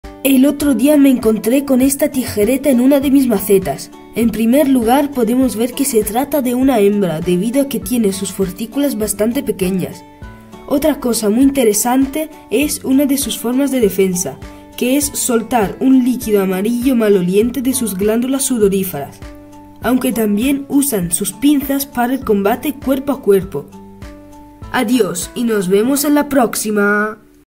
Las tijeretas (forficula auricularia)
Espero que os haya gustado el video, como que no lo dije en el video, las tijeretas no son peligrosas para el ser humano.